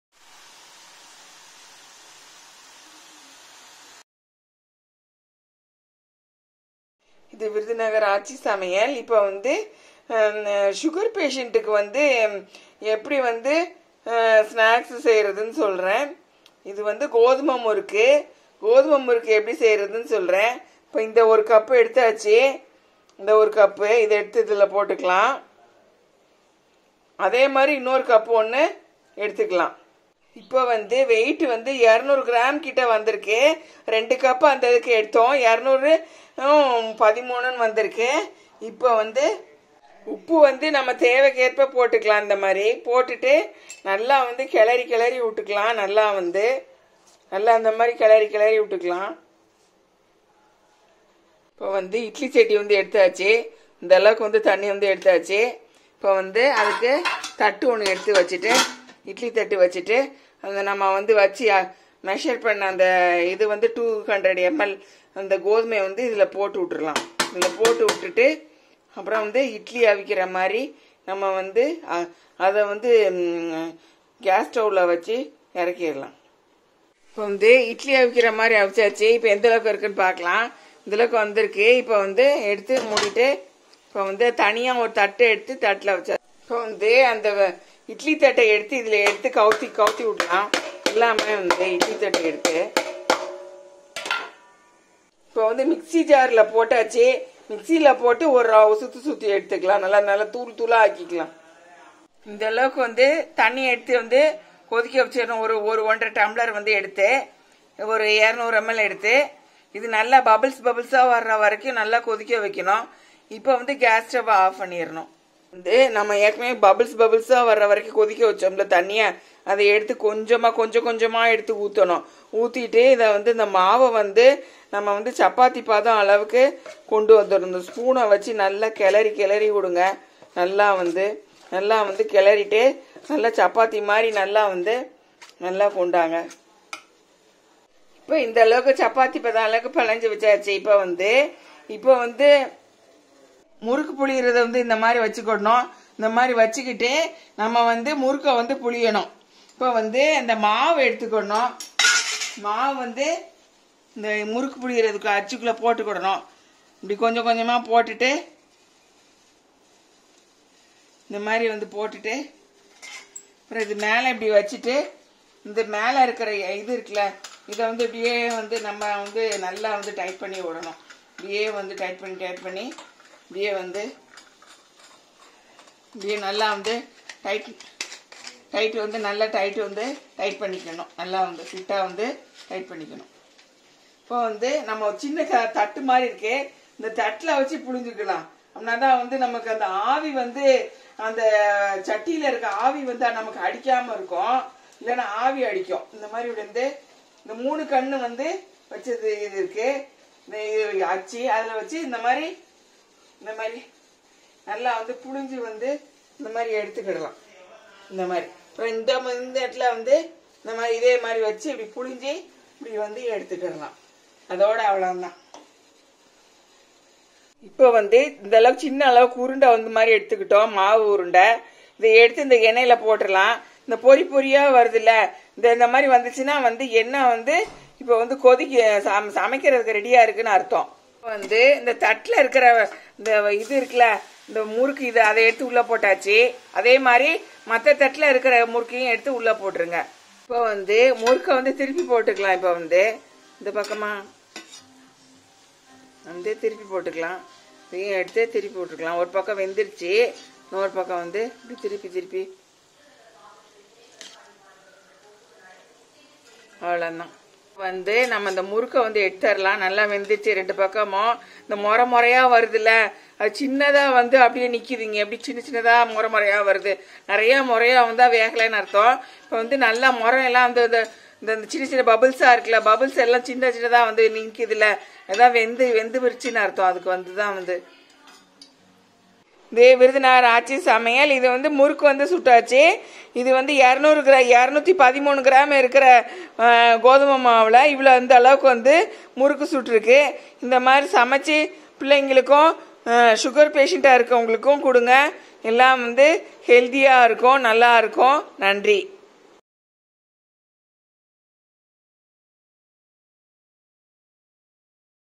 How to cook snacks for diabetic patients? Wheat Murruku.
Very tasty and healthy.